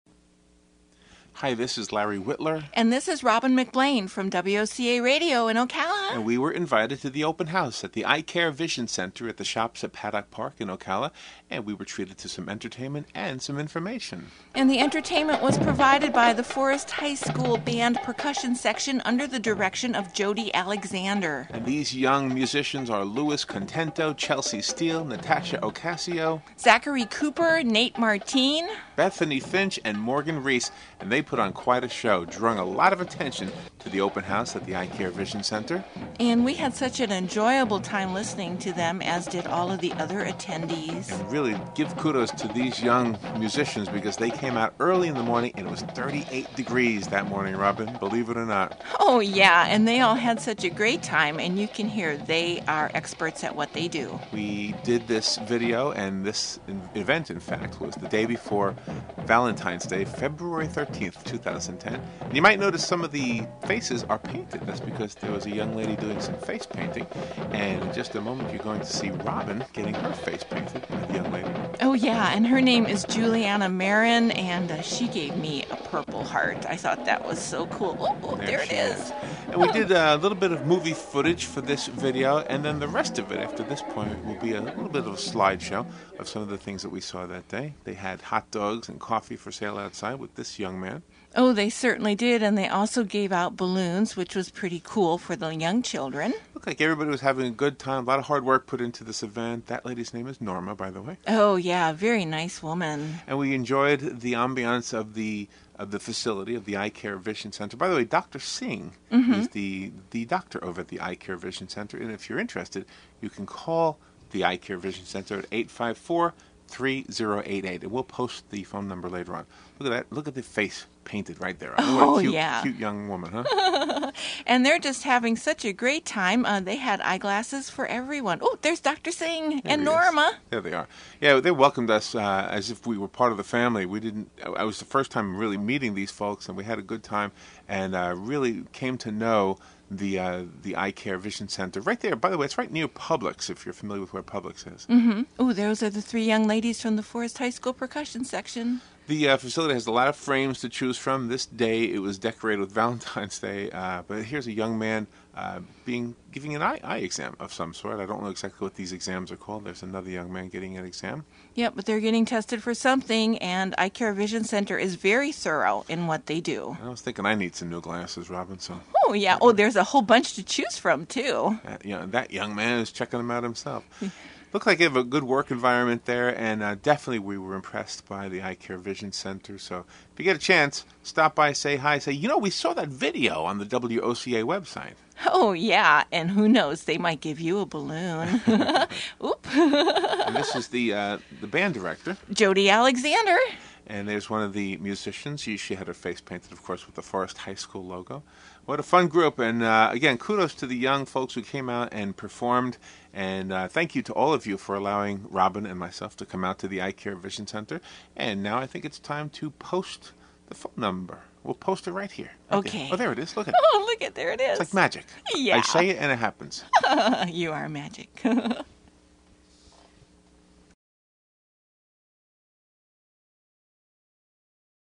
I-Care Vision Center Open House
This is a video and slide show featuring the open house at I-Care
Vision Center in the Shoppes At Paddock Park in Ocala, Florida with
special musical performances by the Forest High School percussion
section.